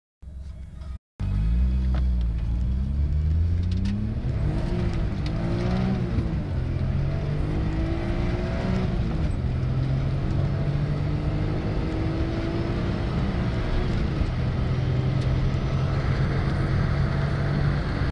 Turbo Mira 0-110KMH 1992 L200s
This is a video of a daihatsu mira trxx doing a 0-110kmh or 0-70mph,